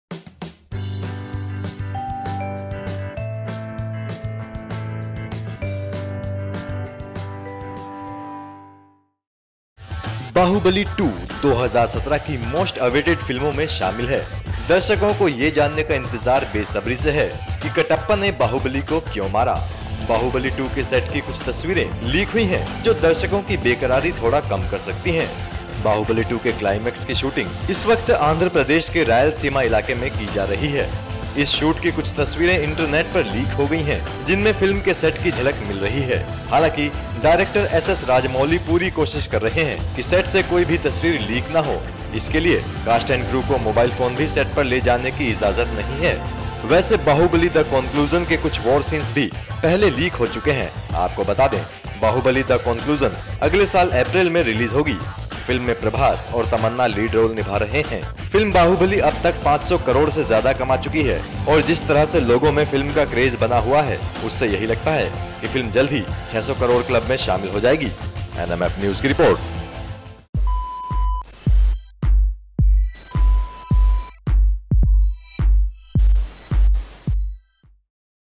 LEAK हुई Baahubali: The Conclusion की EXCLUSIVE तस्वीरे
The shooting of ace filmmaker S S Rajamouli’s Baahubali: The Conclusion is going on in full swing. The director shoulders huge expectations given that the first part of the period drama was a blockbuster that pushed the envelope for the Indian film industry.Not just the director, the entire cast and crew have been giving everything they have as they strive to exceed the standards they set with Baahubali: The Beginning. Sample this: actor Prabhas has trained for about 30 days just to shoot one particular action scene in the film.